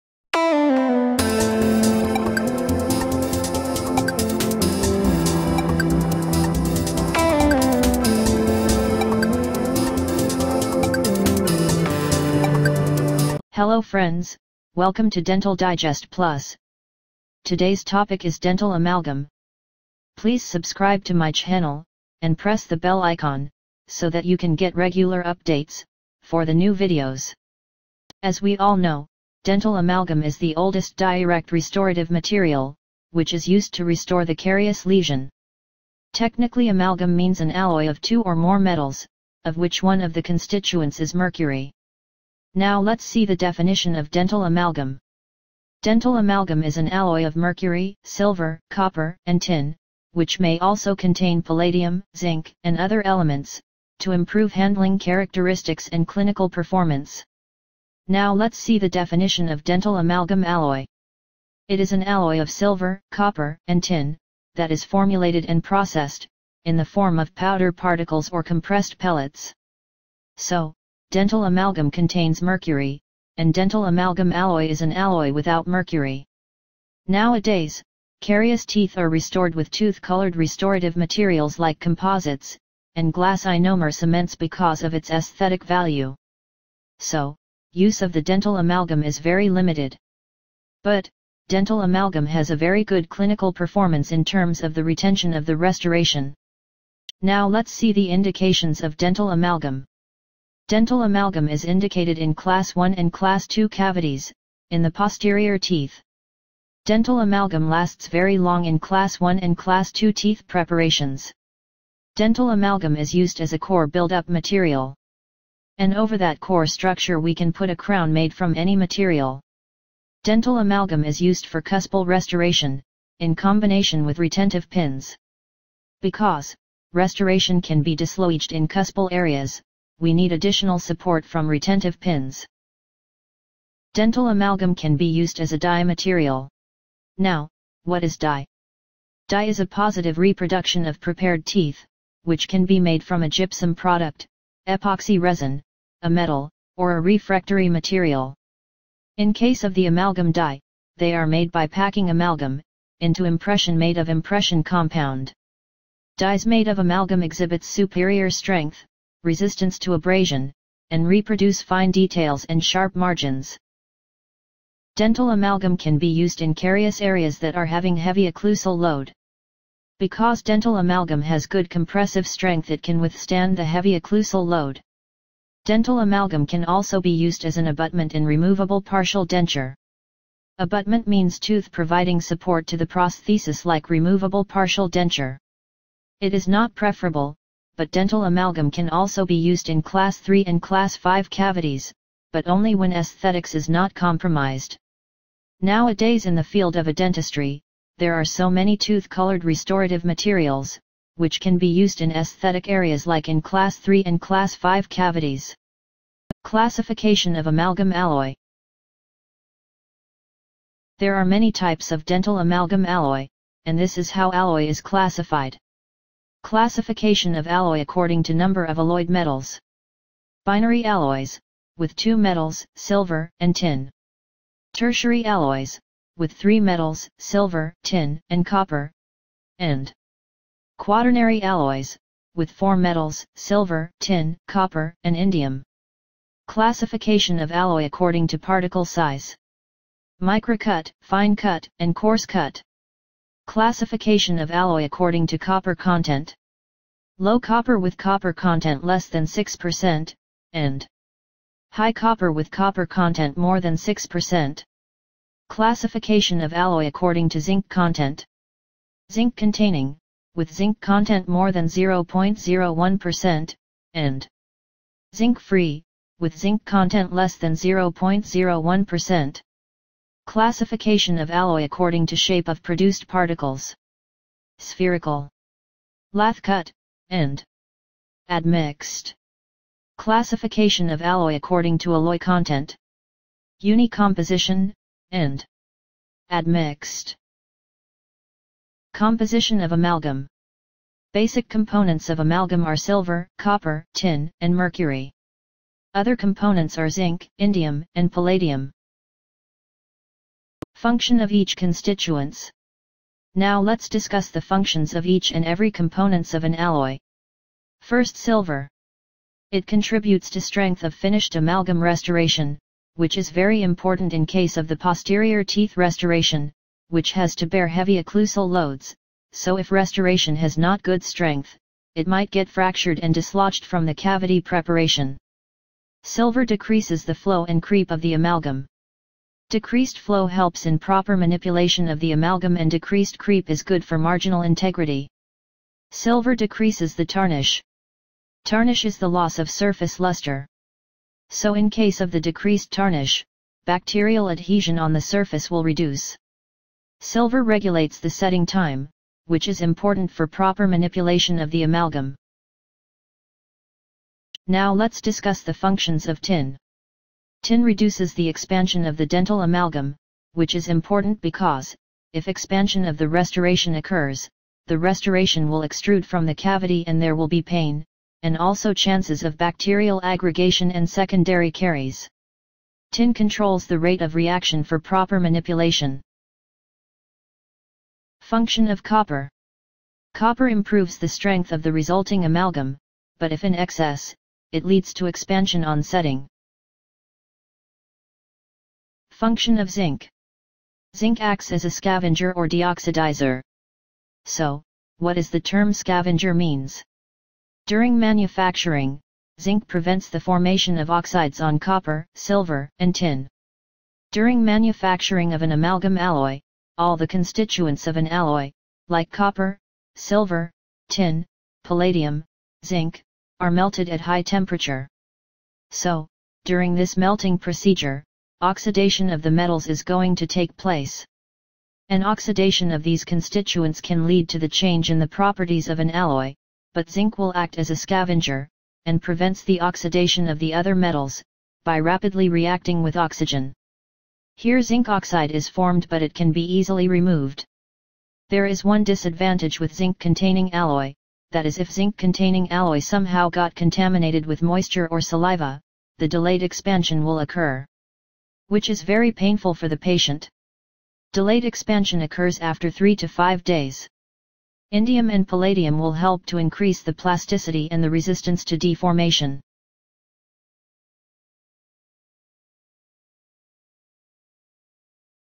Dental Amalgam Part - 1
This video is for better understanding of the basics of dental amalgam.
 In this video you can learn about
Uses of dental amalgam ,
Classification of dental amalgam ,
Composition of dental amalgam ,
Functions of each constituents etc.
Hope this video will provide you good material for better understanding of dental amalgam